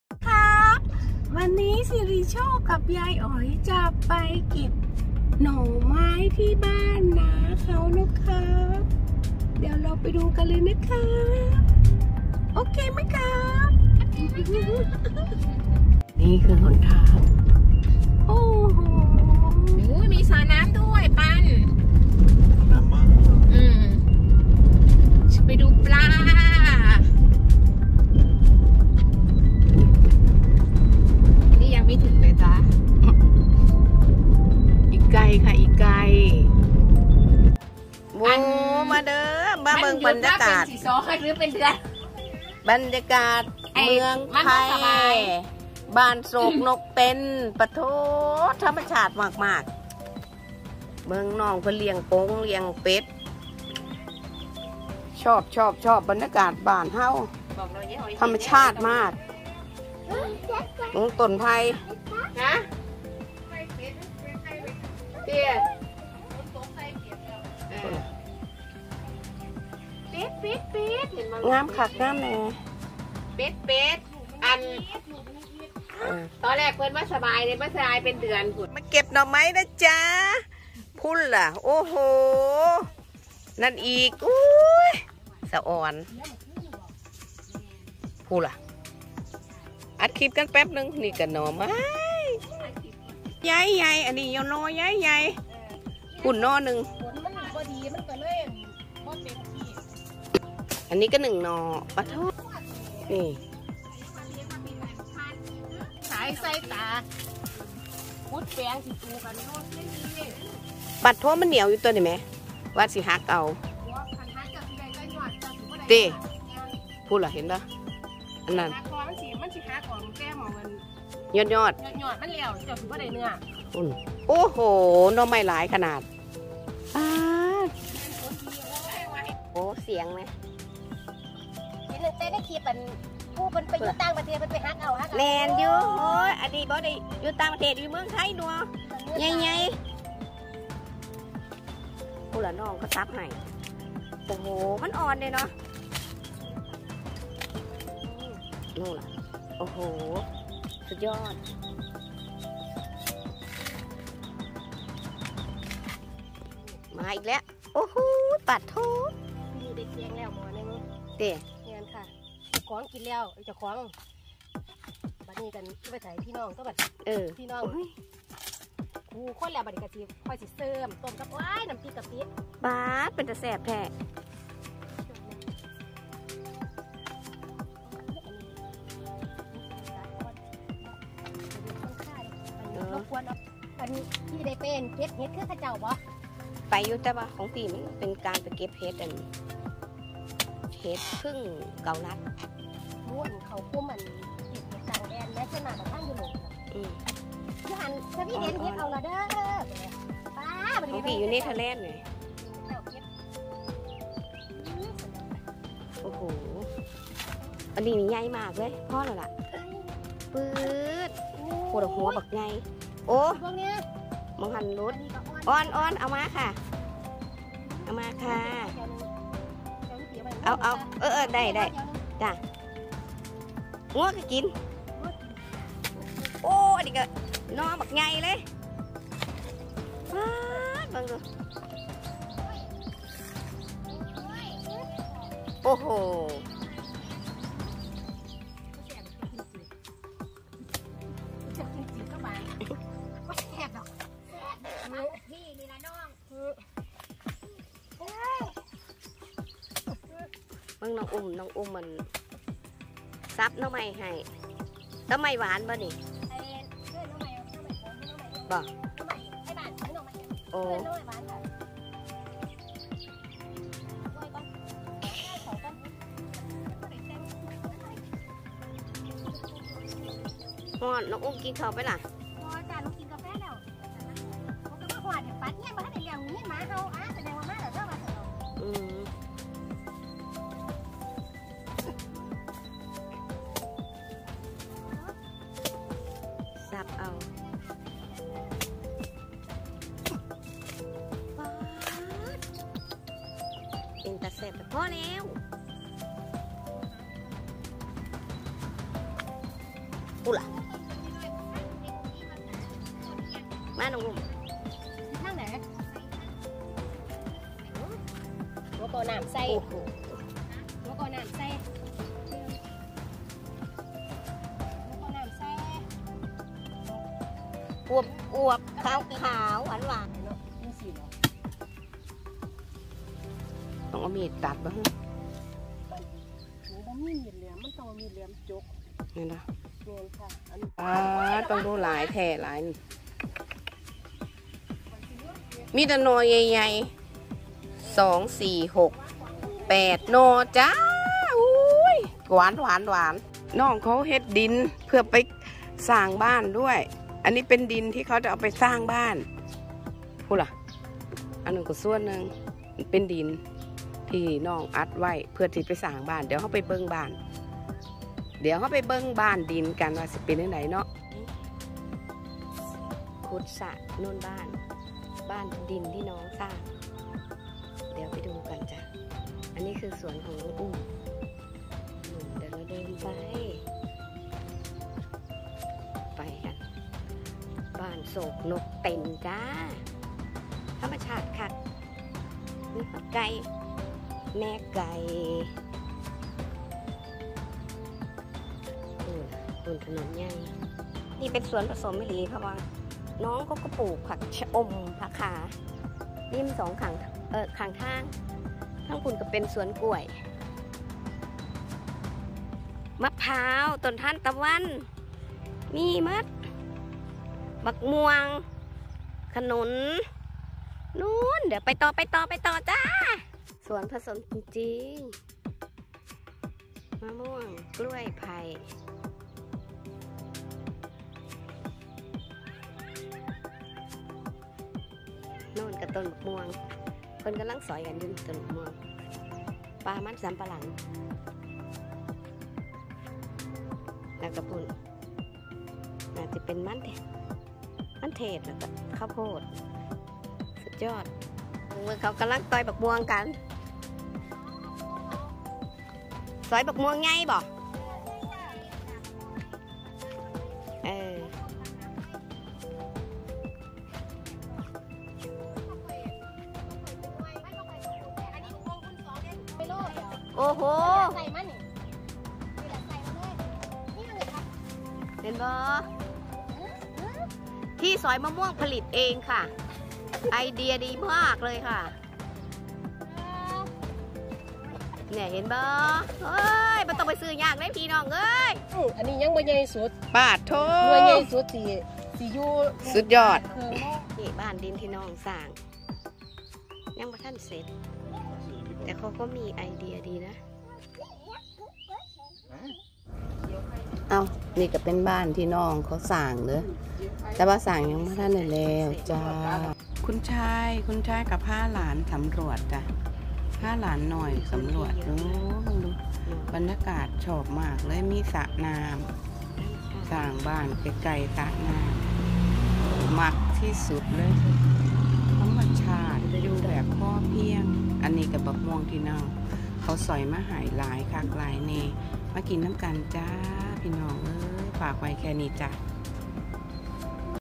เก็บหน่อไม้ชมธรรมชาติและบ้านดิน
เก็บหน่อไม้ชมธรรมชาติและบ้านดิน ช่วงอยู่เมืองไทยมีโอกาสได้ไปเยี่ยมน้องอุ้มบ้านโสกนกเตน น้องสับหน่อไม้/มะม่วงให้ เราก็ได้มีโอกาสชมธรรมชาติบ้านนาที่สวยงามมากจึงนำบรรยากาศมาฝากทุกคนที่ผ่านมาชมค่ะ#happiness